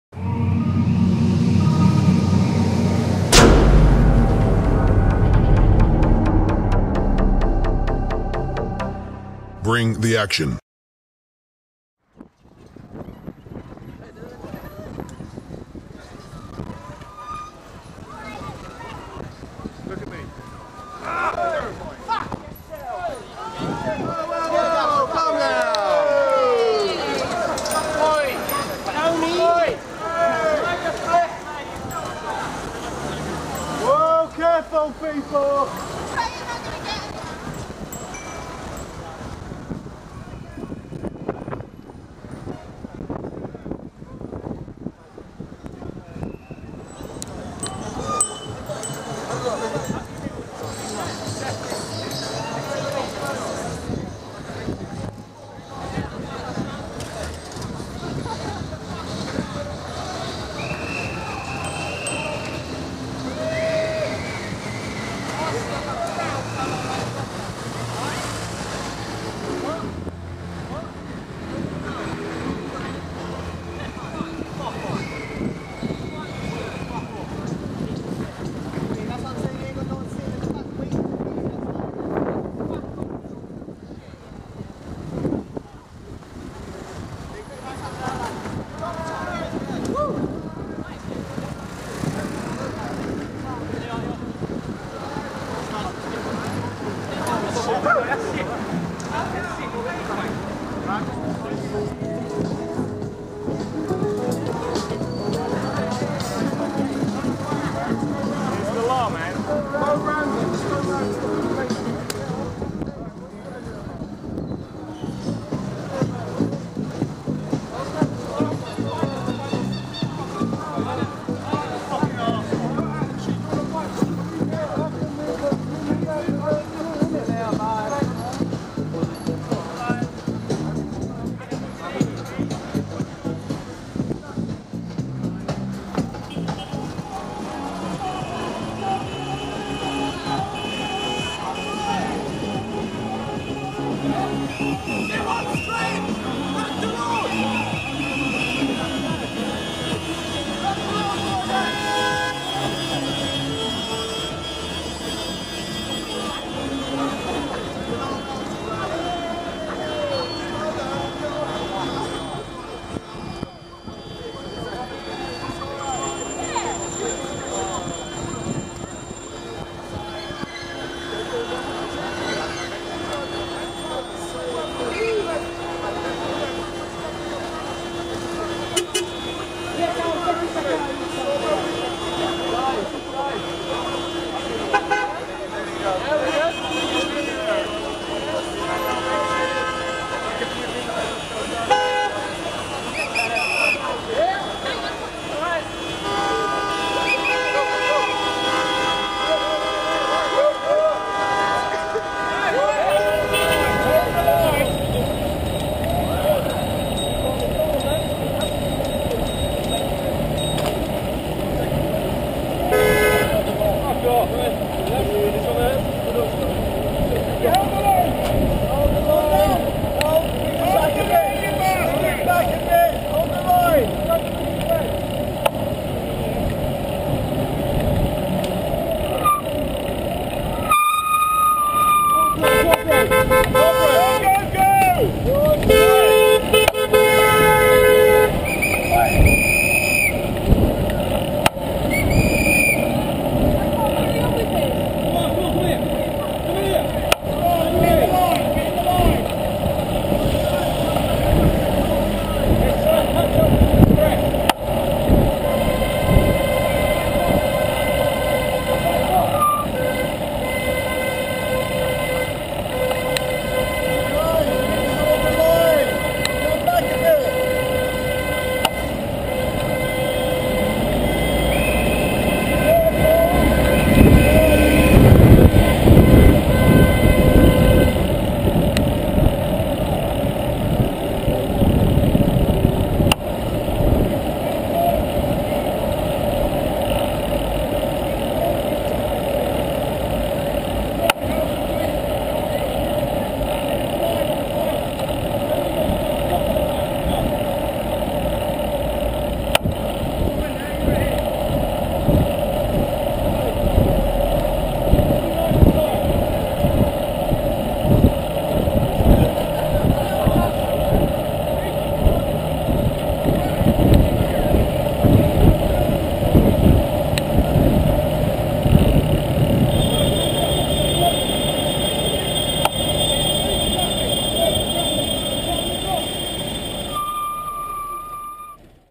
Road Rage At Critical Mass London - Corking (Compilation 2)
Corking: The process of blocking traffic from side roads so that the mass can proceed through red lights freely and quickly without interruption.
0:13 1.- A driver tries to hit cyclists with his vehicle's door and then drives away in anger.
0:48 2.- A taxi driver swears and makes death threats to young cyclist blocking his way.
1:42 3.- Motorcyclist and a bicyclist get engaged in an argument.
2:17 4.- A driver comes out of his car to affront a group of cyclists blocking his way.
3:24 5.- Tail of Critical Mass takes over the whole Park Lane Rd.